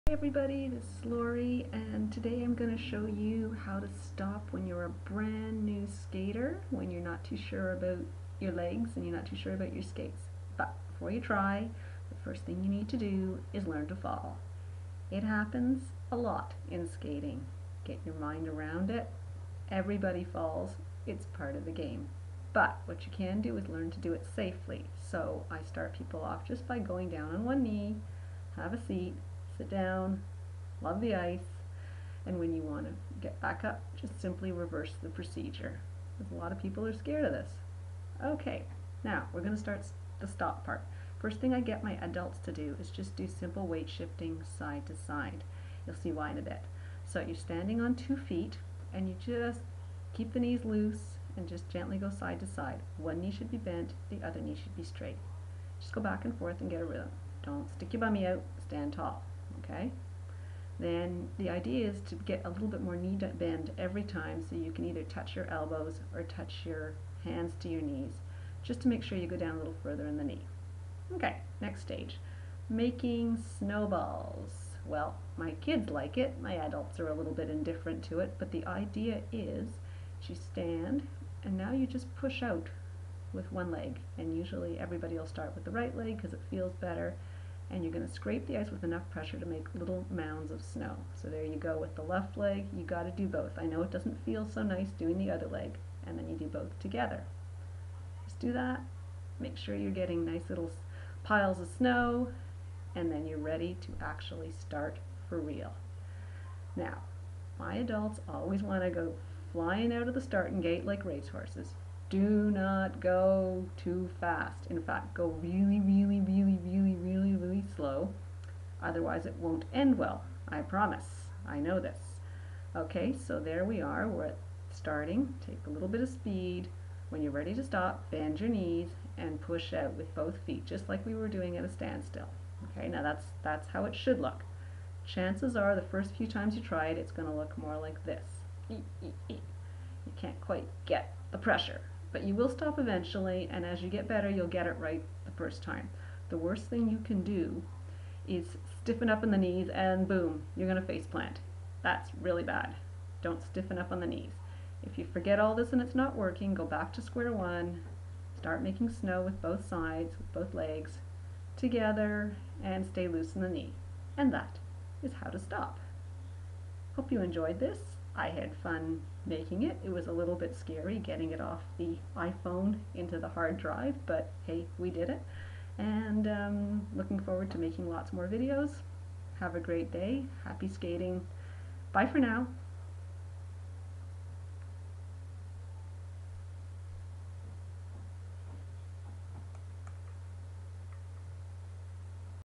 Ice Skating for Absolute Beginners: How to Stop.
for lots more articles and videos.    Sorry about the scary hair...this was shot after two hours of plractice :)